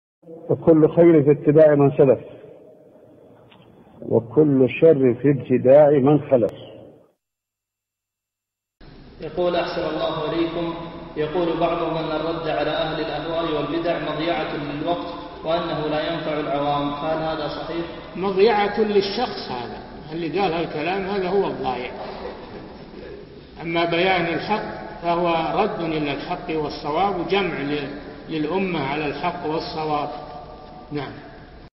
Sind Widerlegungen Zeitverschwendung? | Shaykh Sālih bin Fawzān Al-Fawzān
Frage:
AhsanAllahu Ilaykum, einige Leute sagen: Widerlegungen gegen die Leute der Gelüste und Bid'ah ist Zeitverschwendung und es nützt den 'Awaam (einfachen Muslimen) nicht, ist diese Aussage wahr?

Shaykh Sālih bin Fawzān Al-Fawzān:
Es ist eine Verschwendung für diese Person; der der diese Aussage geäußert hat ist der Verlierer!Was die Wahrheit aufzuklären betrifft, so it es zur Wahrheit und Richtigkeit zurückzukehren und ein Mittel für die Einigkeit der Ummah auf der Wahrheit & Richtigkeit. Na'am